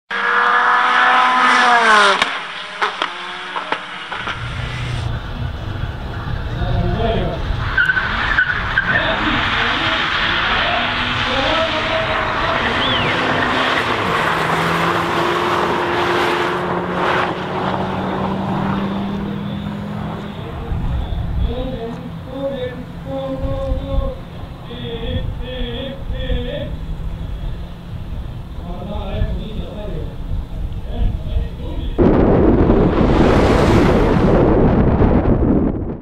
Mika Soras Seat Cupra 1km: 248km/h 1 Mile: 273km/h
HRX One Mile Challenge 2018 Round 1